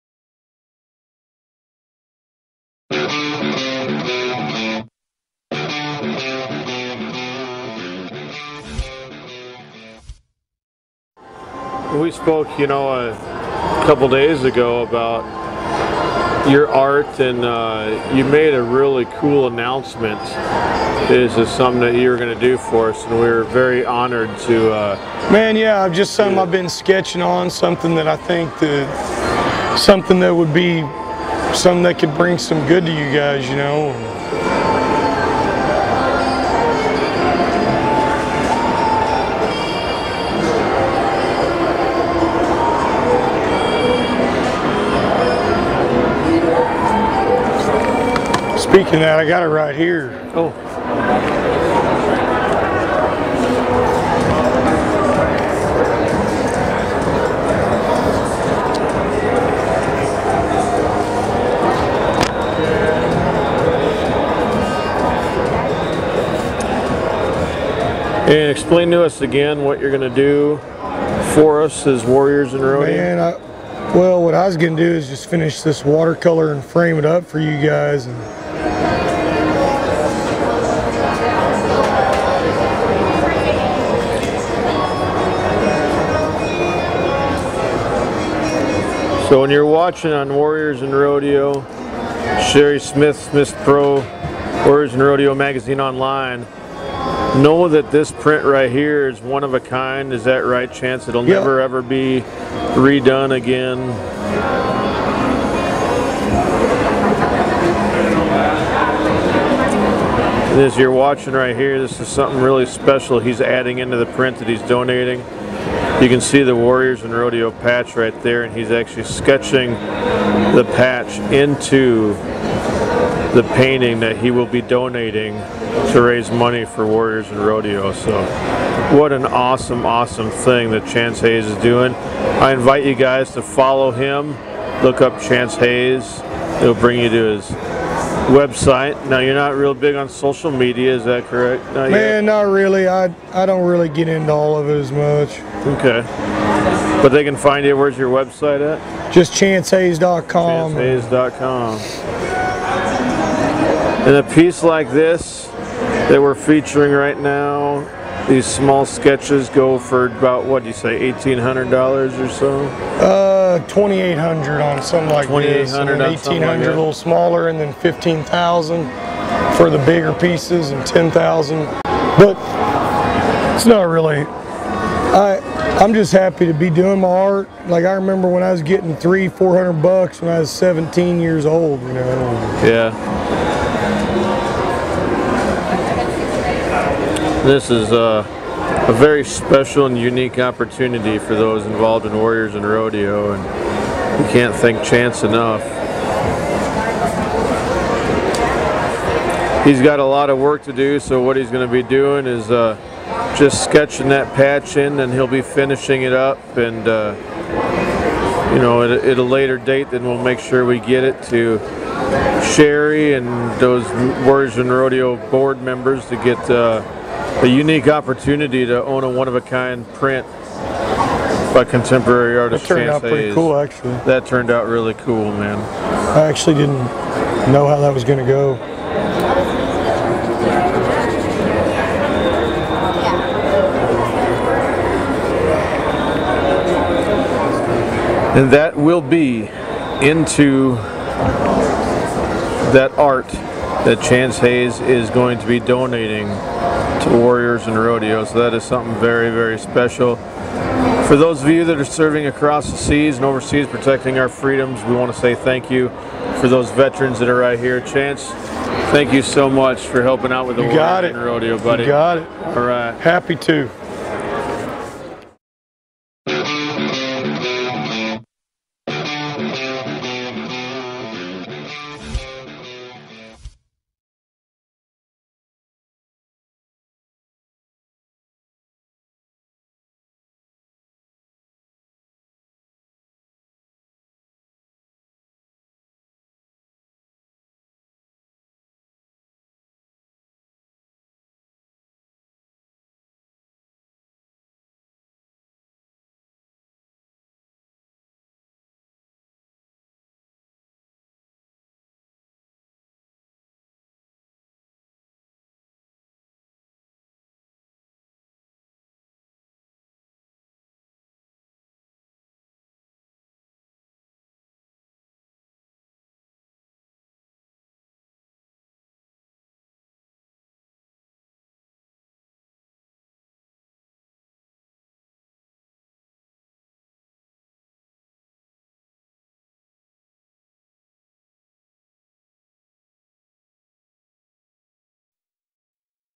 The Rodeo Roundup Has A Special Announcement With Artist Chance Hays
While out in Las Vegas covering the 2017 Wrangler National Finals Rodeo the Rodeo Round Up's Jason Hetland also had the honor of representing Warriors and Rodeo.  While talking to contemporary artist Chance Hays about the organization he wanted to make a special annoncement that he wanted us to share with all of you

▬ CONNECT ▬

▬ SUPPORT ▬